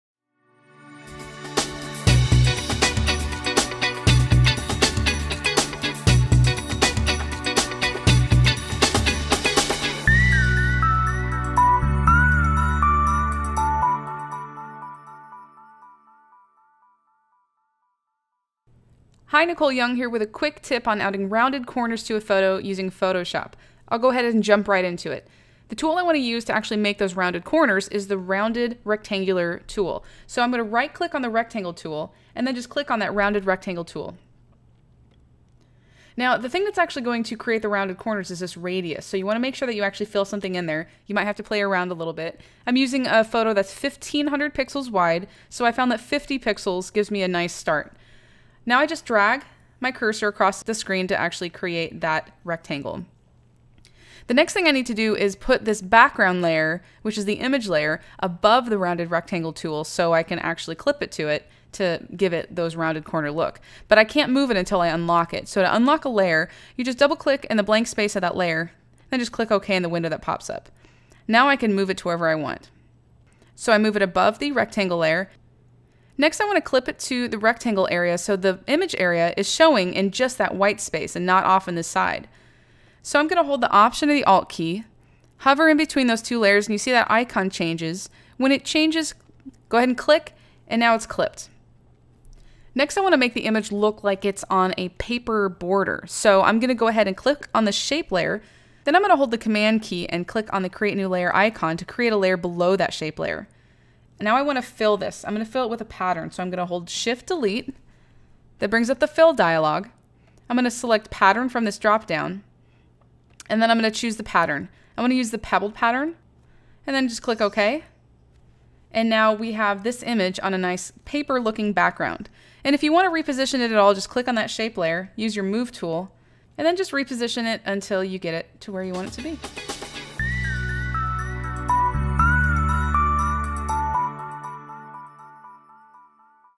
Two Minute Tip: Creating Rounded Corners in Photoshop
In this tutorial, I demonstrate how to add rounded corners to an image using Photoshop.